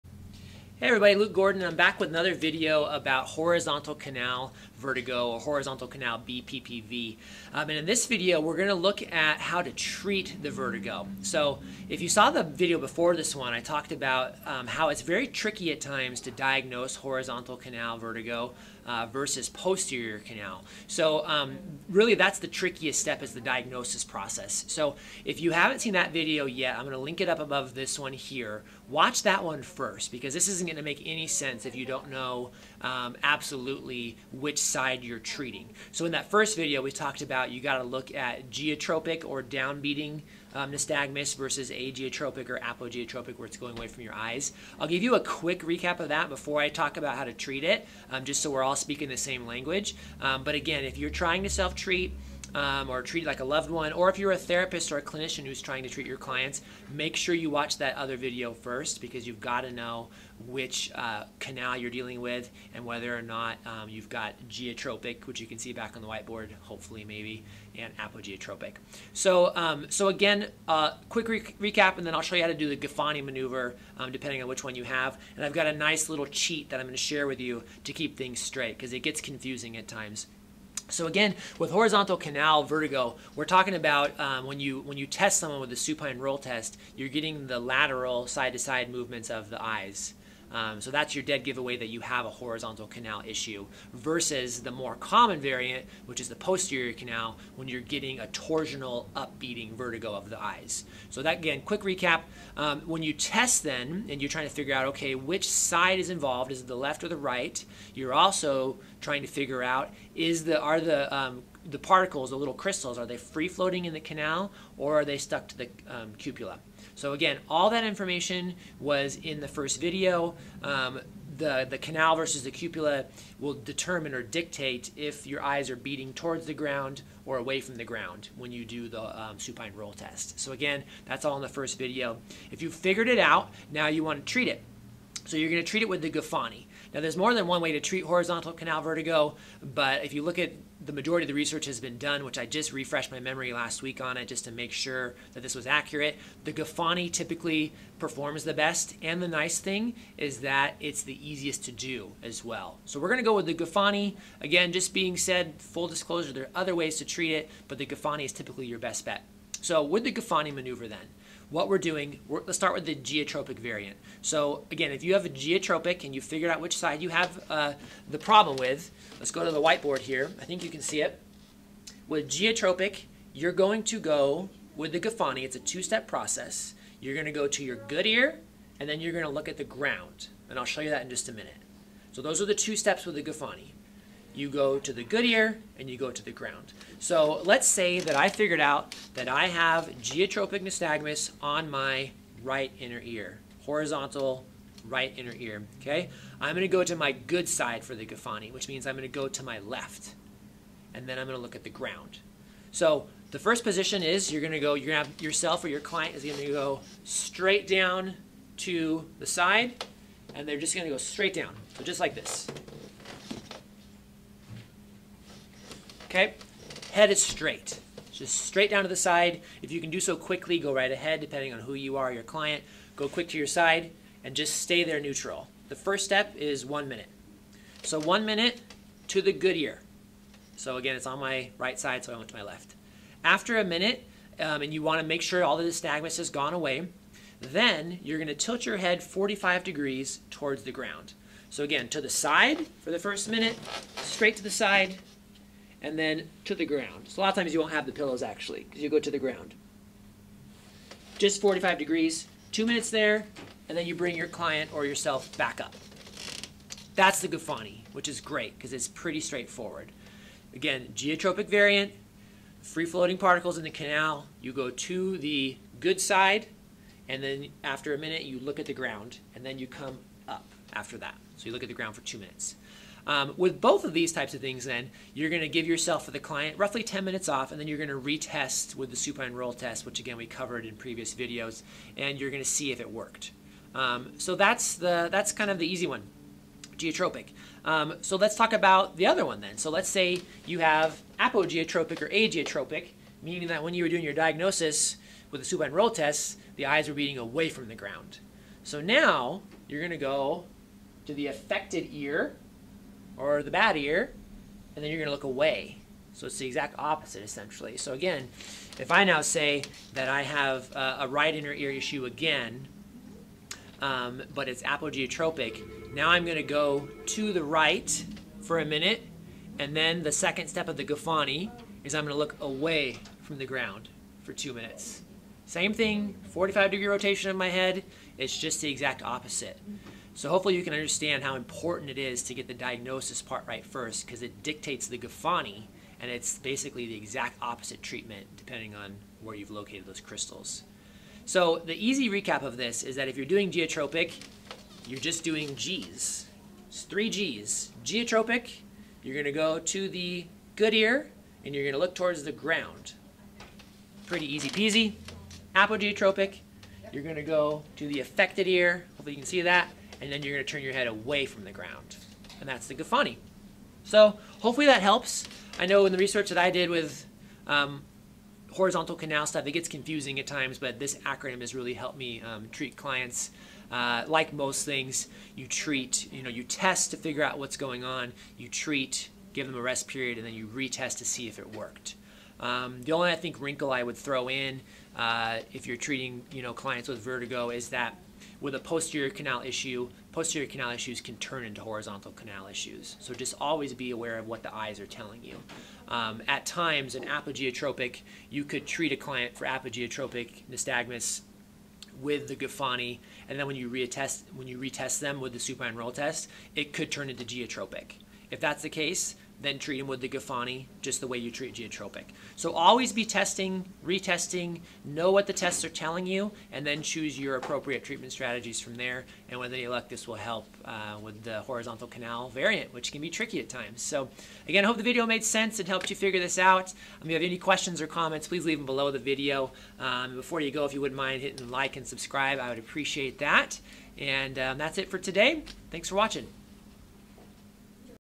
How to Treat Horizontal Canal BPPV (Vertigo) (Gufoni Maneuver)
Once you've figured out that you have horizontal canal BPPV, and you've figured out which side is involved (right vs left), and you've figured out if you have geotropic or apogeotropic nystagmus (indicating canalithiasis vs. cupulolithiasis), you can now progress to treatment!

Wow, that was a lot of big words for one sentence... if that didn't make sense to you, make sure you check out my previous video about how to diagnose horizontal canal BPPV first by clicking this

The best and easiest treatment option for horizontal canal BPPV (both types) is the Gufoni Maneuver.

In this video, I'll show you how to perform the 2 versions of the Gufoni Maneuver to treat both types of horizontal canal BPPV (geotropic/canalithiasis and apogeotropic/cupulolithiasis).

I hope it helps you treat horizontal canal BPPV!

- Luke Gordon, DPT
(Doctor of Physical Therapy/Owner of Gordon Physical Therapy)